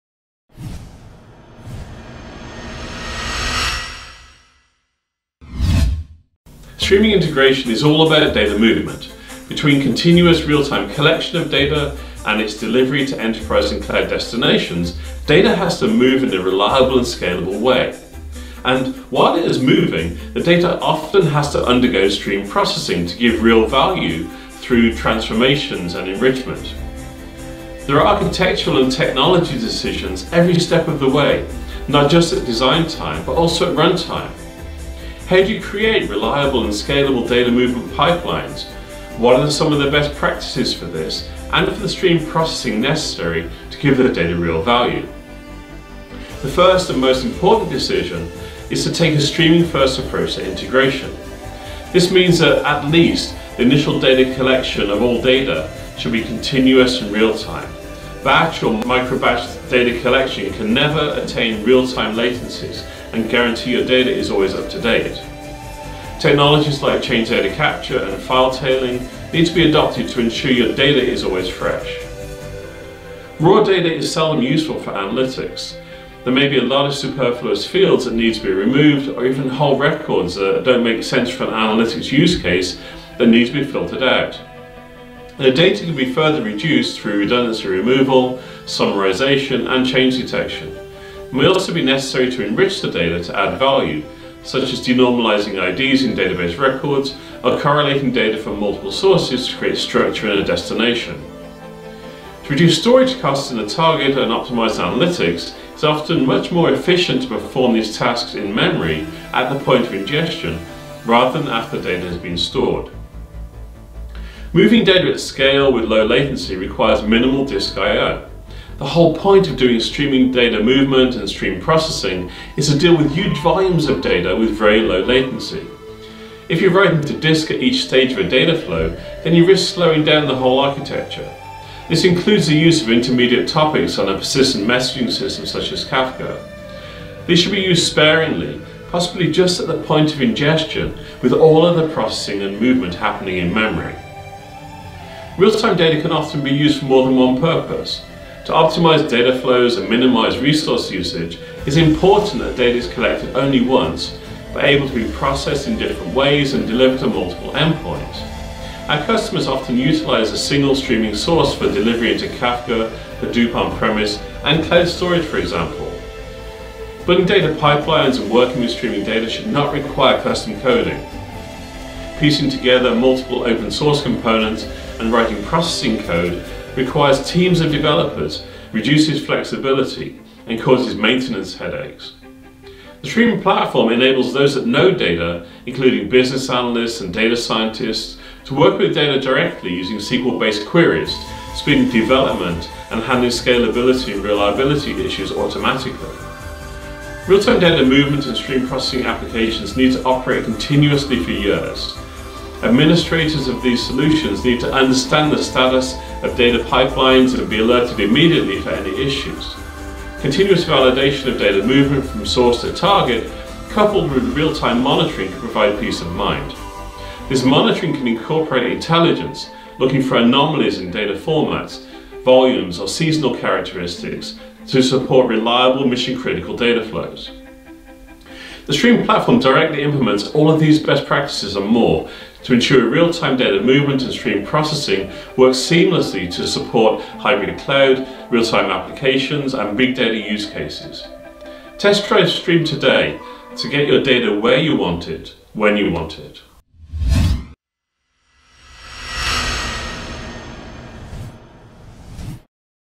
Best Practices for Real-Time Data Movement and Stream Processing
What are some of the best practices for real-time data movement, and for the stream processing necessary to give the data real value?

Streaming integration is all about data movement. Between continuous real-time collection of data, and its delivery to enterprise and cloud destinations, data has to move in a reliable and scalable way. And, while it is moving, the data often has to undergo stream processing to give it real value through transformations and enrichment. There are architectural and technology decisions every step of the way – not just at design time, but also at run time.

So what are the best practices for real-time data movement? How do you create reliable and scalable data movement pipelines?

1.  Take a Streaming-First approach
The first, and most important decision is to take a streaming first approach to integration. This means that at least the initial collection of all data should be continuous and real-time. Batch or microbatch data collection can never attain real-time latencies and guarantee your data is always up-to-date. Technologies like change data capture (CDC), and file tailing need to be adopted to ensure your data is always fresh.

2.  Raw data is seldom useful for analytics
There may be a lot of superfluous fields that need to be removed, or even whole records that don’t make sense for an analytics use case, that need to be filtered out. The data can be further reduced through redundancy removal, summarization, and change detection. It may also be necessary to enrich the data to add value, such as denormalizing IDs in database records, or correlating data from multiple sources to create structure in a destination. To reduce storage costs in the target, and optimize analytics, it is often much more efficient to perform these tasks in-memory at the point of ingestion, rather than after the data has been stored.

3.  Moving data at scale, with low latency, requires minimal disk I/O
The whole point of doing real-time data movement and stream processing is to deal with huge volumes of data with very low latency. If you are writing to disk at each stage of a data flow, then you risk slowing down the whole architecture. This includes the use of intermediate topics on a persistent messaging system such as Kafka. These should be used sparingly, possibly just at the point of ingestion, with all other processing and movement happening in-memory.

4.  Real-time streaming data can often be used for more than one purpose
To optimize data flows, and minimize resource usage, it is important that this data is collected only once, but able to be processed in different ways and delivered to multiple endpoints. Our customers often utilize a single streaming source for delivery into Kafka, Hadoop on-premises, and cloud storage, simultaneously and in real time.

5.  Custom coding should not be required
Building data pipelines and working with streaming data should not require custom coding. Piecing together multiple open source components, and writing processing code requires teams of developers, reduces flexibility, and causes maintenance headaches. The Striim platform enables those that know data, including Business Analysts and Data Scientists, to work with the data directly using SQL-based queries, speeding development and handling scalability and reliability issues automatically.

6.  Real-time processing operates continuously
Real-time data movement and stream processing applications need to operate continuously for years. Administrators of these solutions need to understand the status of data pipelines and be alerted immediately for any issues. Continuous validation of data movement from source to target, coupled with real-time monitoring, can provide peace of mind. This monitoring can incorporate intelligence, looking for anomalies in data formats, volumes, or seasonal characteristics to support reliable mission-critical data flows.

The Striim platform directly implements all of these best practices, and more, to ensure real-time data movement and stream processing best support hybrid cloud, real-time applications, and fast data use cases.